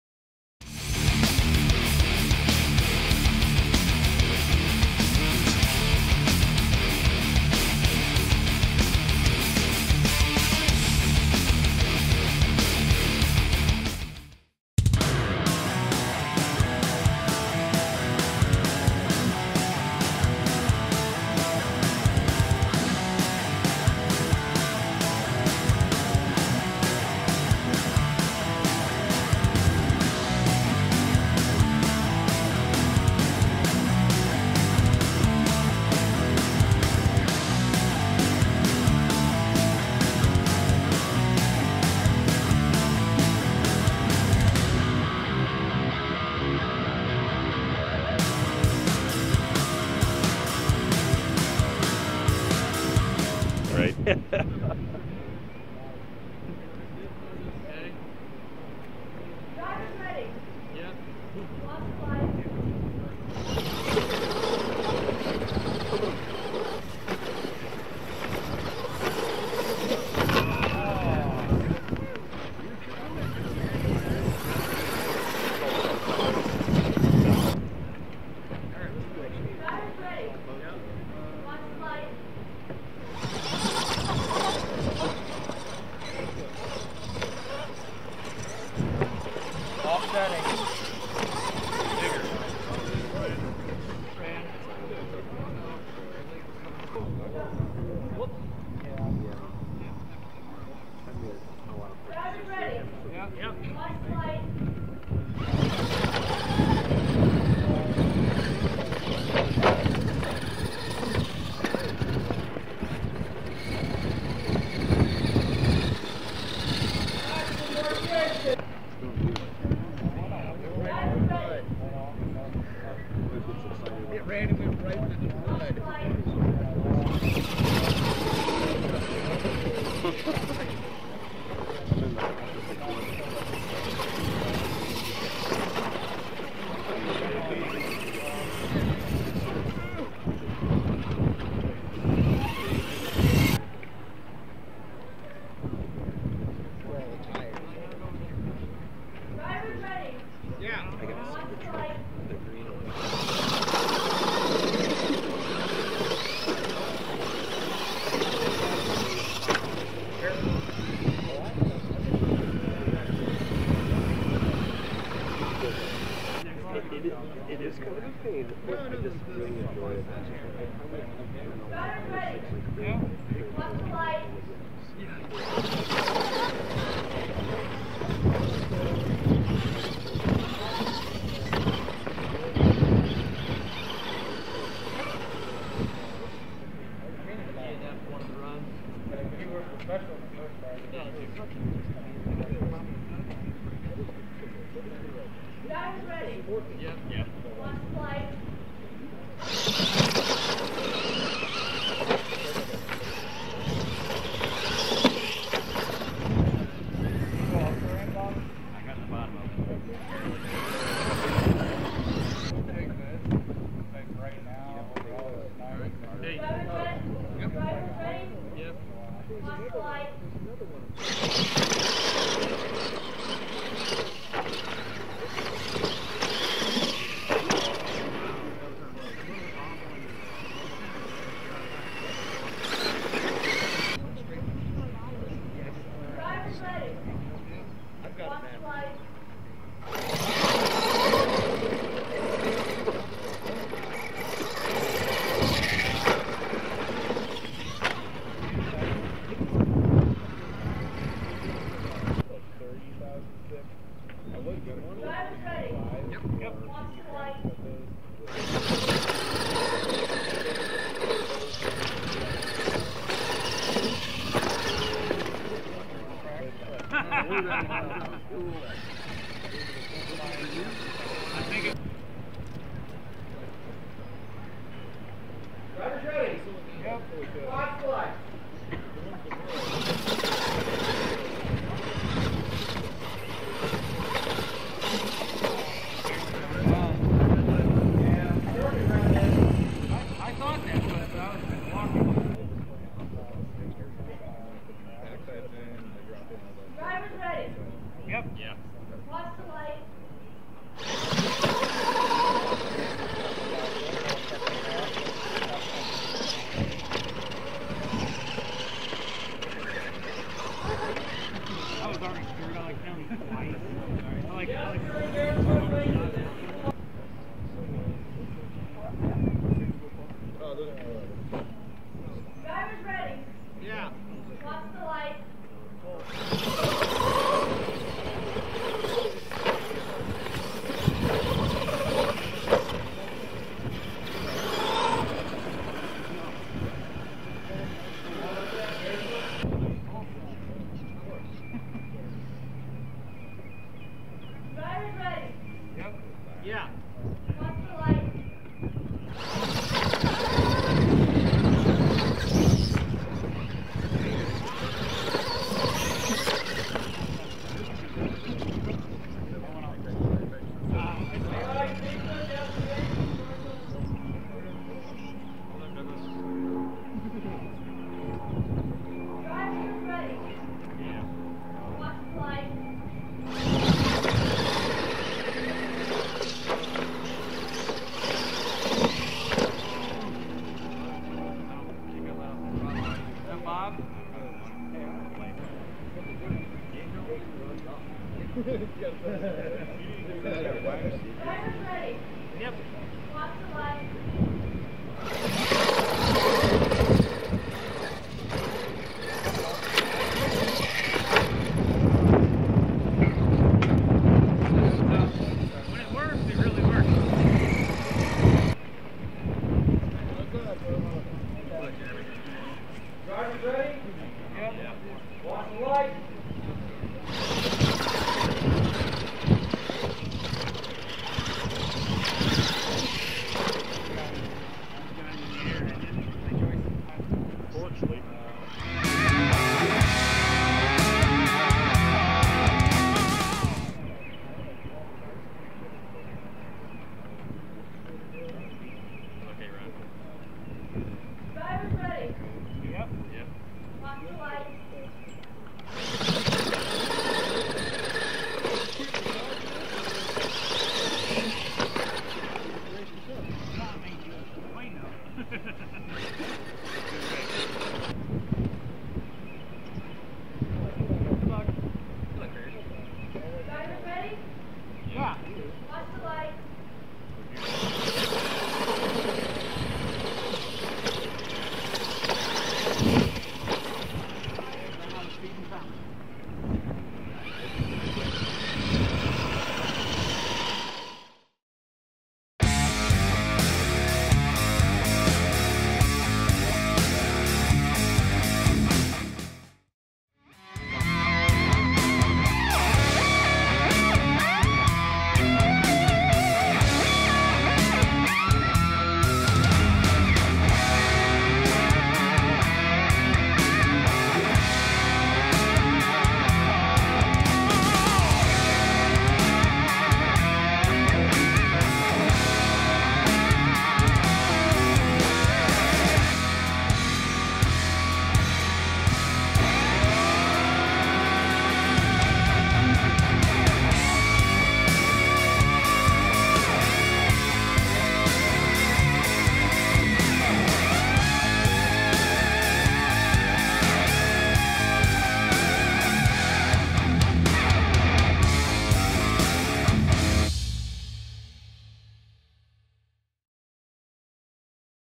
2023 Opening Weekend Sport Mod Brkt 2 - Trigger King R/C
2023 opening weekend Sport Mod racing bracket from April 2nd at jbscalegraphics.com Raceway.